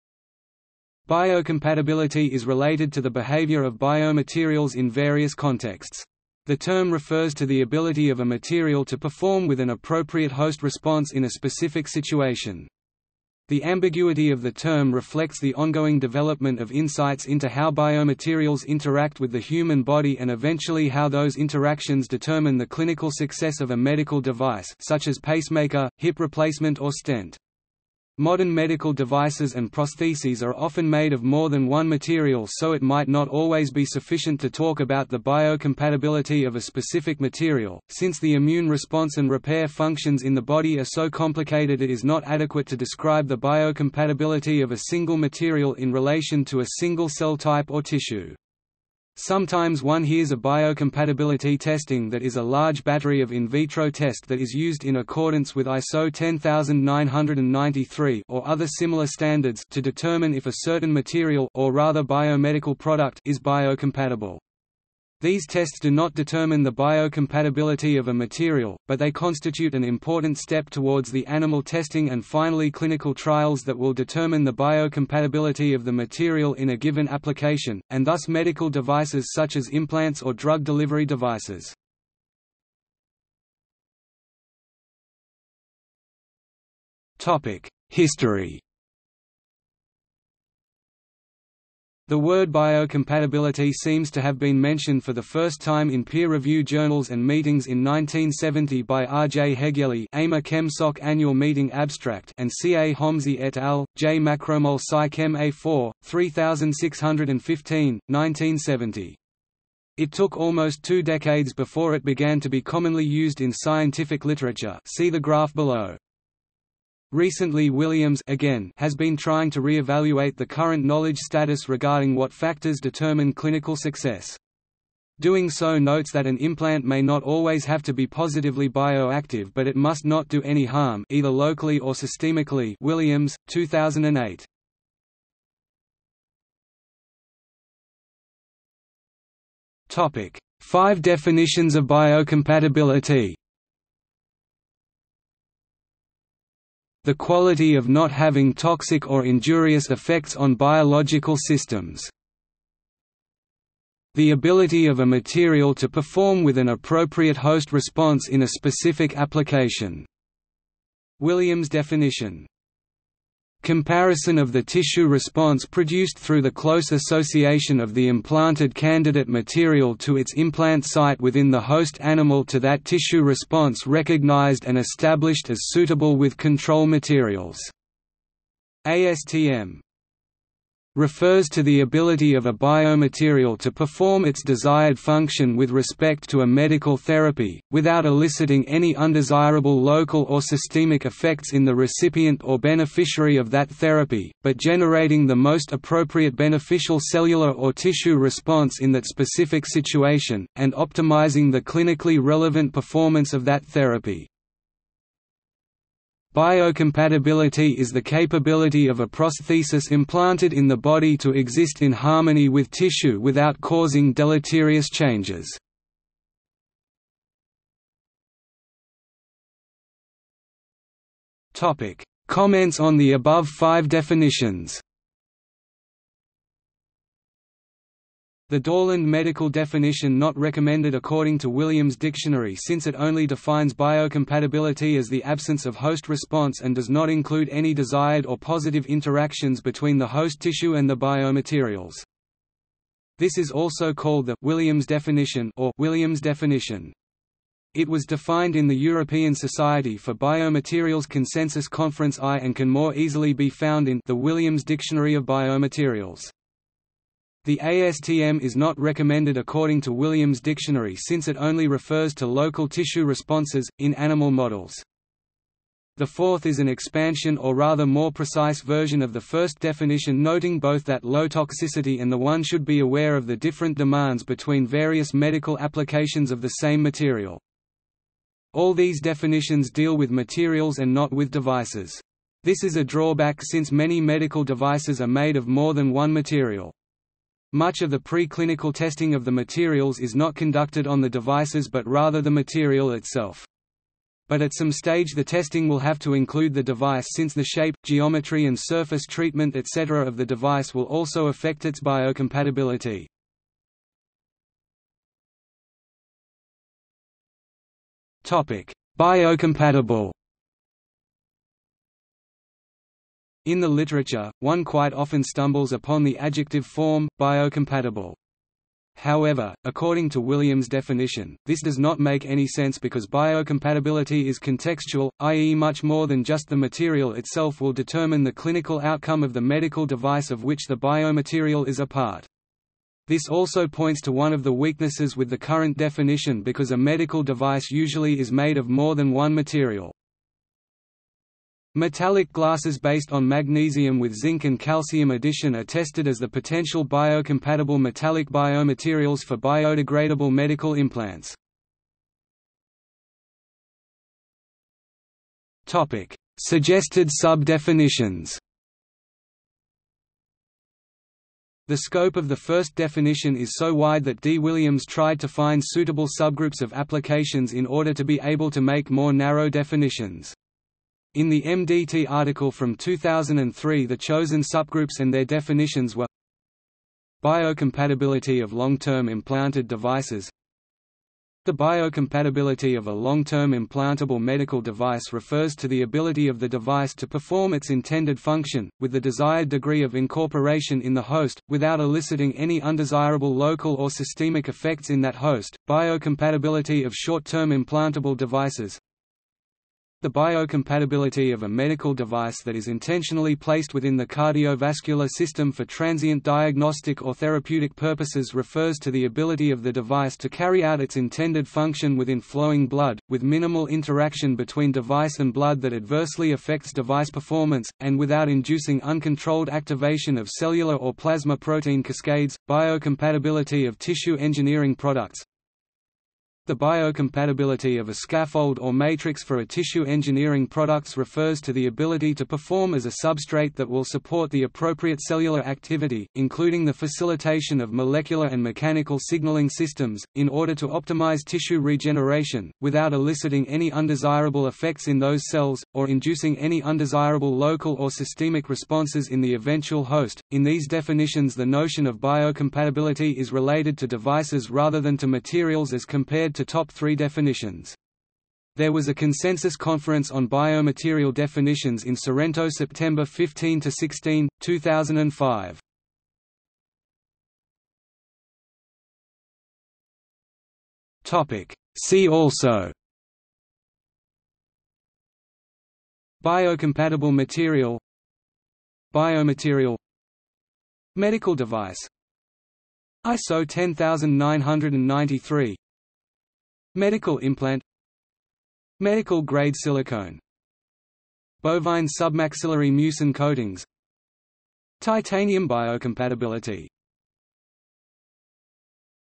Biocompatibility | Wikipedia audio article
00:01:23 1 History
00:02:16 2 Five definitions of biocompatibility
00:03:38 3 Comments on the above five definitions
00:05:05 3.1 ‘Biocompatible’
00:05:52 3.2 Suggested sub-definitions
00:07:47 4 See also

- Socrates

SUMMARY
Biocompatibility is related to the behavior of biomaterials in various contexts. The term refers to the ability of a material to perform with an appropriate host response in a specific situation.  The ambiguity of the term reflects the ongoing development of insights into how biomaterials interact with the human body and eventually how those interactions determine the clinical success of a medical device (such as pacemaker, hip replacement or stent). Modern medical devices and prostheses are often made of more than one material so it might not always be sufficient to talk about the biocompatibility of a specific material.Since the immune response and repair functions in the body are so complicated it is not adequate to describe the biocompatibility of a single material in relation to a single cell type or tissue. Sometimes one hears of biocompatibility testing that is a large battery of in vitro test that is used in accordance with ISO 10993 (or other similar standards) to determine if a certain material (or rather biomedical product) is biocompatible. These tests do not determine the biocompatibility of a material, but they constitute an important step towards the animal testing and finally clinical trials that will determine the biocompatibility of the material in a given application, and thus medical devices such as implants or drug delivery devices.